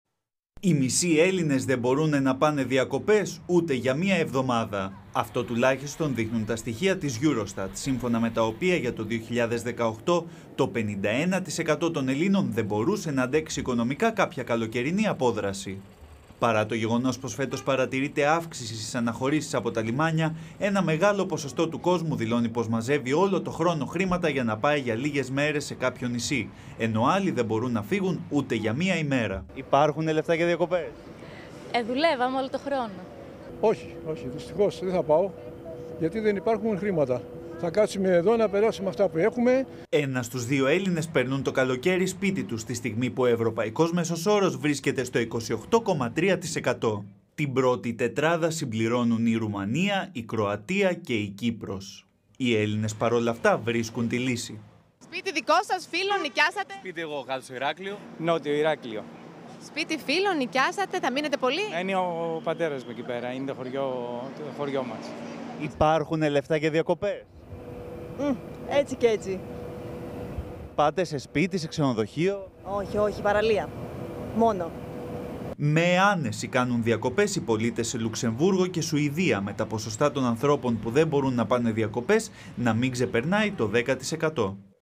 Έρευνα Eurostat: Οι μισοί Έλληνες δεν μπορούν να πάνε διακοπές ούτε μία εβδομάδα
Οι μισοί Έλληνες δε μπορούν να πάνε διακοπές ούτε για μία εβδομάδα. Αυτό - τουλάχιστον - δείχνουν τα στοιχεία της Eurostat, σύμφωνα με τα οποία για το 2018 το 51% των Ελλήνων δε μπορούσε να αντέξει οικονομικά κάποια καλοκαιρινή απόδραση. Παρά το γεγονός πως φέτος παρατηρείται αύξηση στις αναχωρήσεις απο τα λιμάνια, ένα μεγάλο ποσοστό του κόσμου δηλώνει πως μαζεύει όλο το χρόνο χρήματα για να πάει για λίγες ημέρες σε κάποιο νησί, ενώ άλλοι δε μπορούν να φύγουν ούτε για μία ημέρα. 1 στους 2 Έλληνες περνούν το καλοκαίρι σπίτι τους, τη στιγμή που ο ευρωπαϊκός μέσος όρος βρίσκεται στο 28,3%. Τη πρώτη πεντάδα συμπληρώνουν η Ρουμανία (58,9%), η Κροατία (51,3%) και η Κύπρος (51%). Με άνεση κάνουν διακοπές οι πολίτες σε Λουξεμβούργο και Σουηδία με τα ποσοστά των ανθρώπων που δεν μπορούν να πάνε διακοπές να μη ξεπερνά το 10%. Οι Έλληνες παρόλα αυτά βρίσκουν τη λύση.